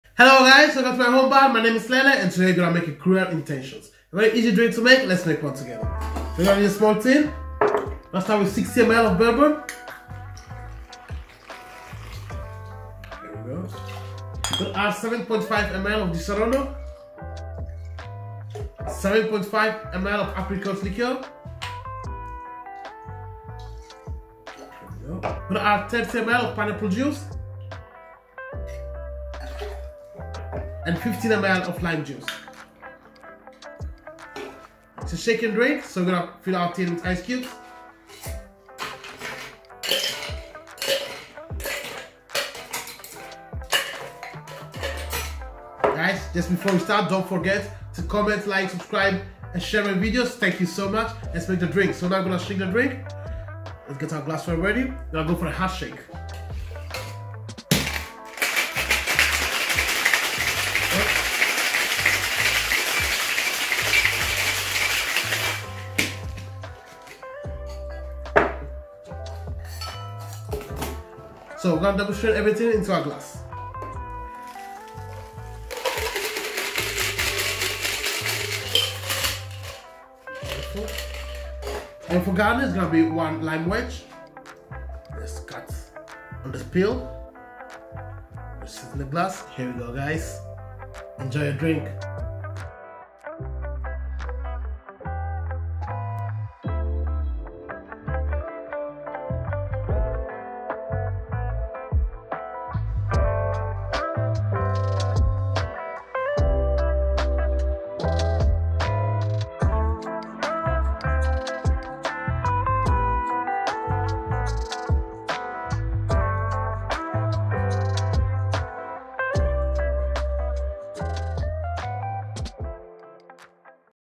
How to make a CRUEL INTENTION with LELE | COCKTAIL with BOURBON
📗 MY NEW BOOK "MEZCAL and TEQUILA Cocktails"

INGREDIENTS:
60ml Bourbon
7.5ml Disaronno Amaretto
7.5ml Apricot Liqueur
30ml Pineapple Juice
15ml Lime Juice

Garnish:
1 Lime Wedge